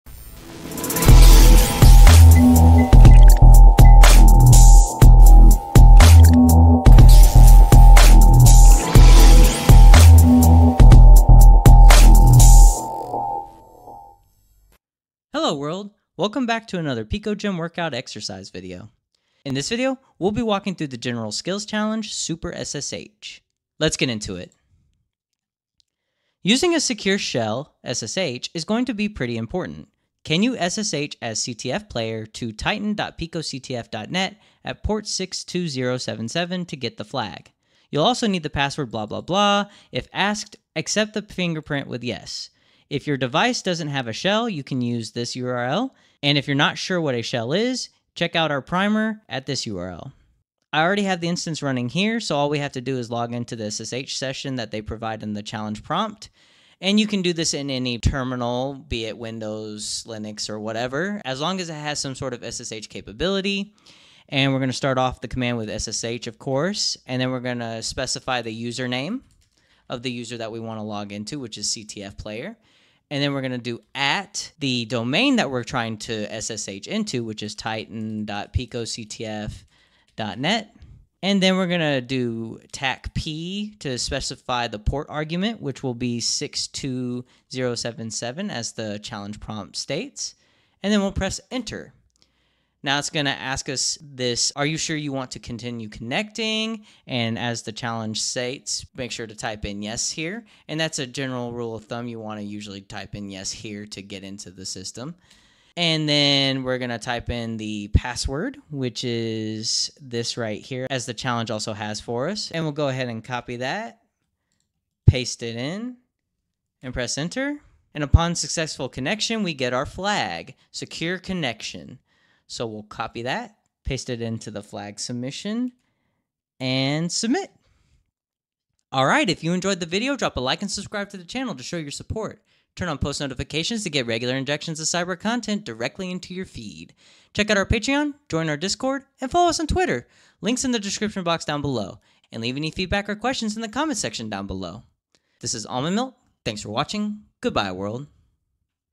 picoGym (picoCTF) Exercise: Super SSH
In this picoGym (picoCTF) Workout video, we do a writeup of the Super SSH general skills challenge.